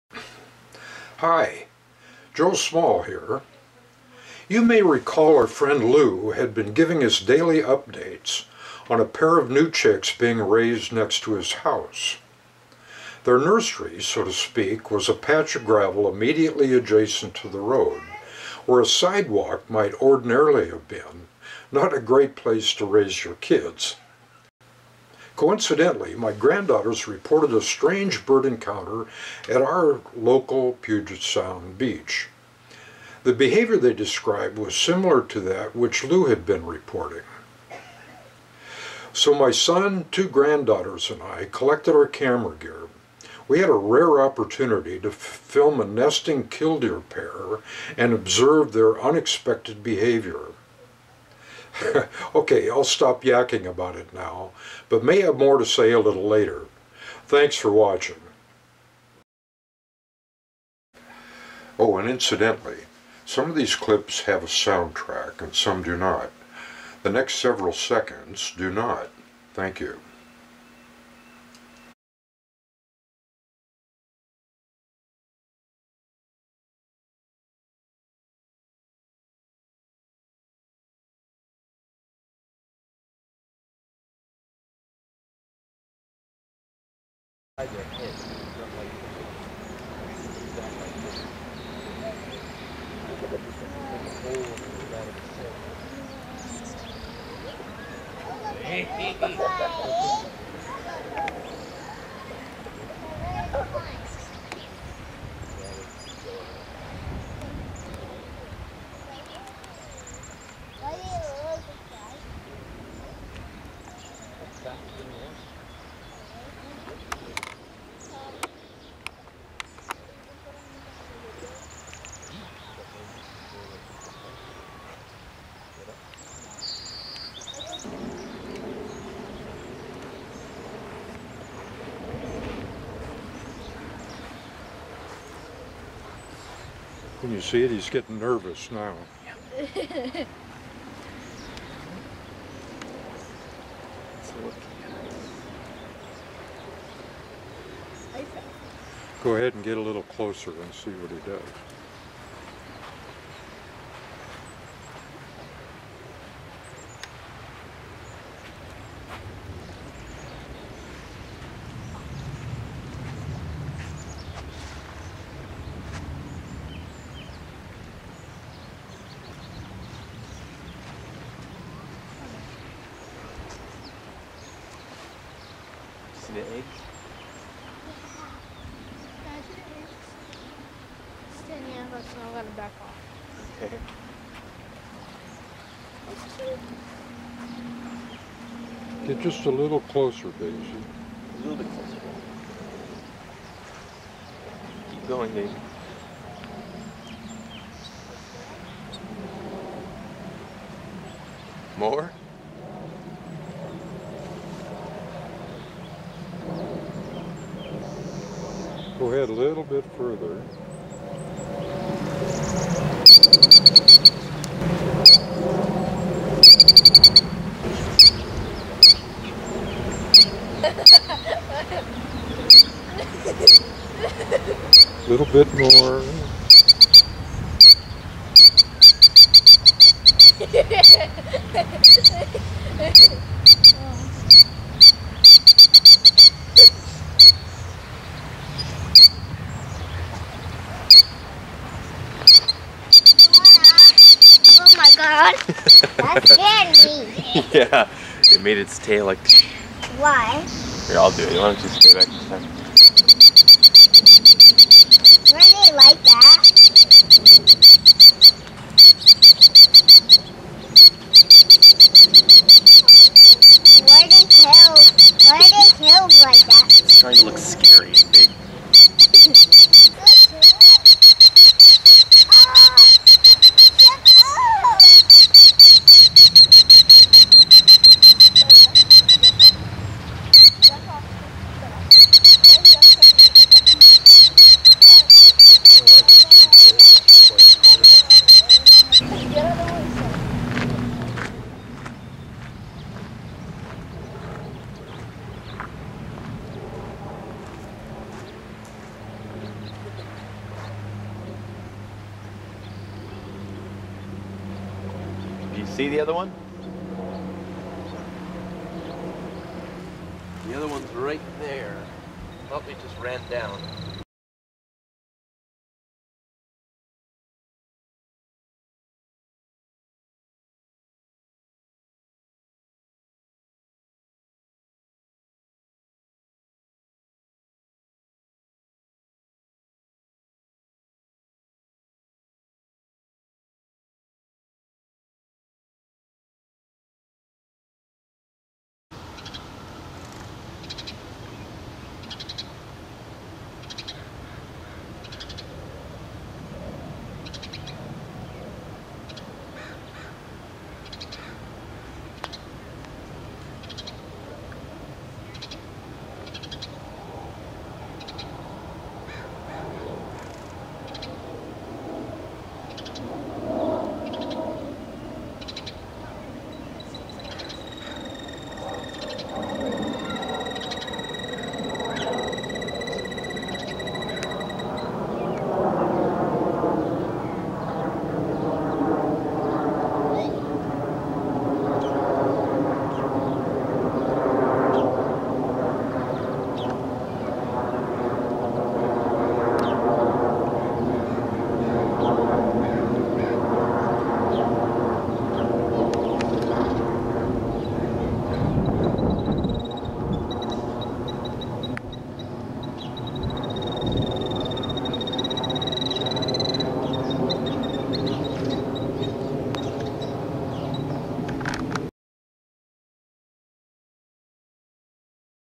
Killdeer (Charadrius vociferus)
Breeding Killdeer are rare in our neighborhood.  A handsome pair nesting in an unusual and possibly hazardous location.